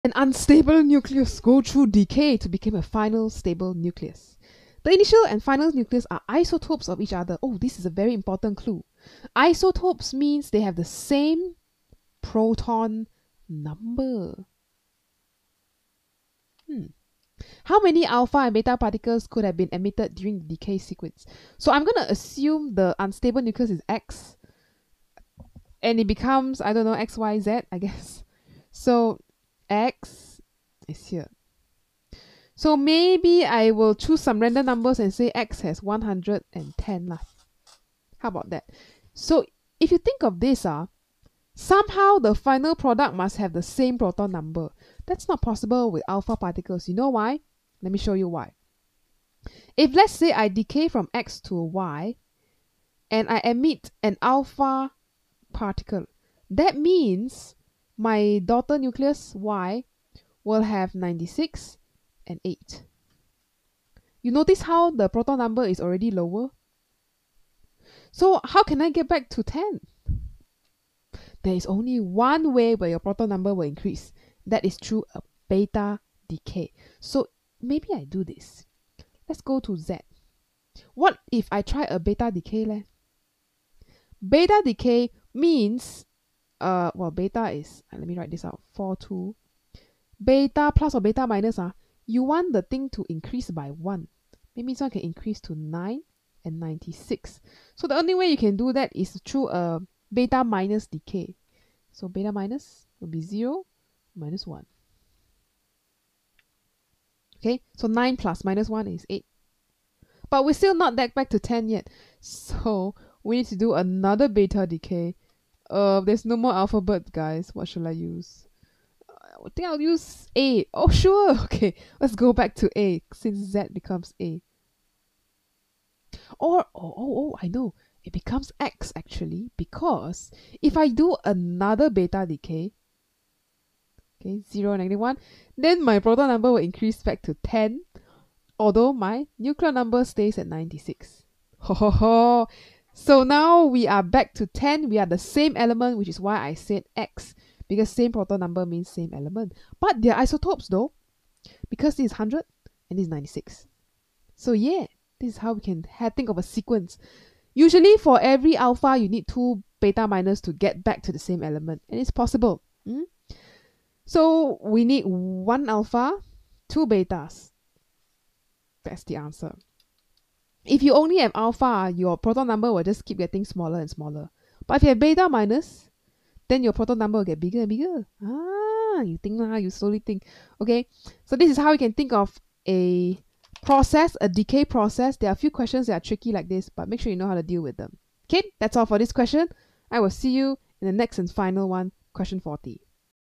MJ20 P12 Q39 Decay Chain into Isotope | May/June 2020 | CAIE A Level 9702 Physics
9702/12/M/J/20: An unstable nucleus goes through successive decays to become a final, stable nucleus. The initial nucleus and the final nucleus are isotopes of each other. How many α and β– particles could have been emitted during the decay sequence?

9702_s20_qp12